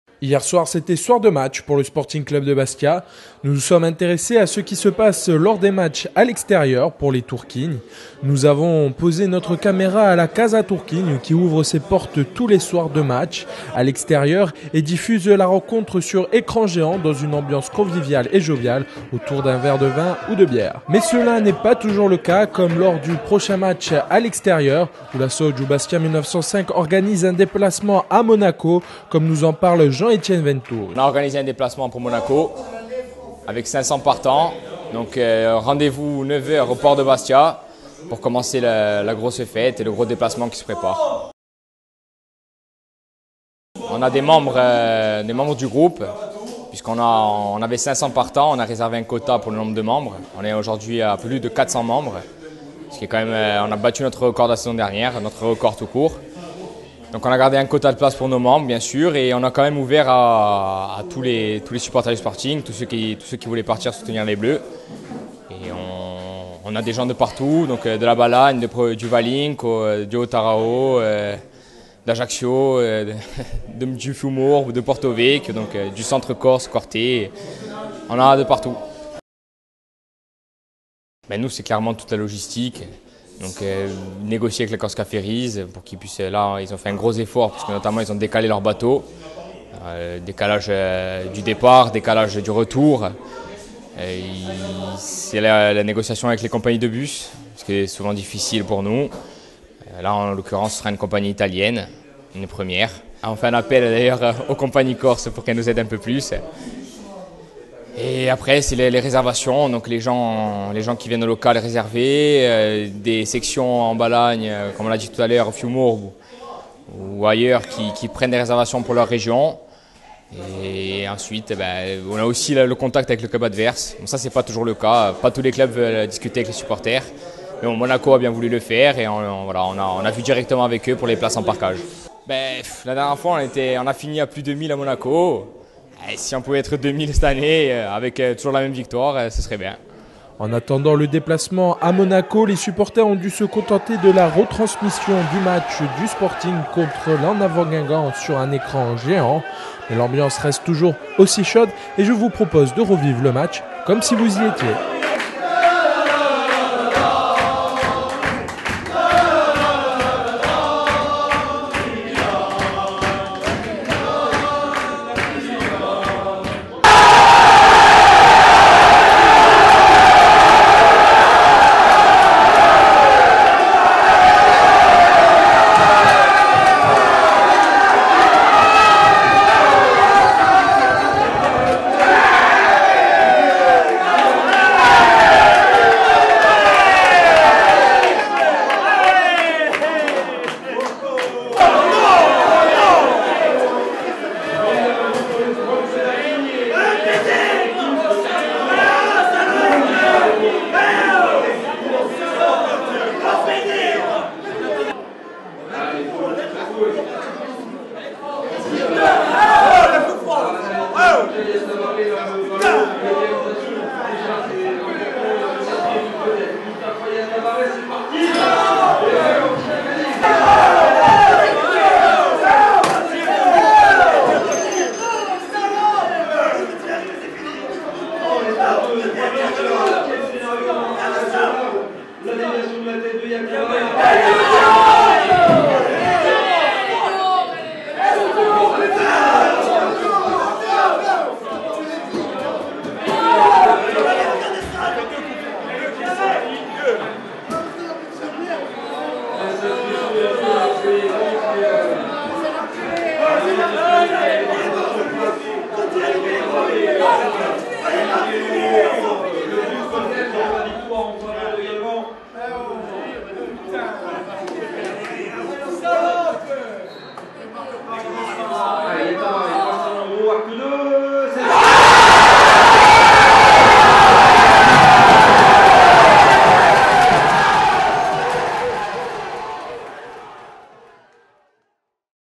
L'ambiance les soirs de match a la Casa Turchina
Interview de Jean-Etienne Venturi sur l'organisation du déplacement à Monaco par Bastia 1905 et l'ambiance de soir de match à la Casa Turchina.

Reportage : Cyril Martino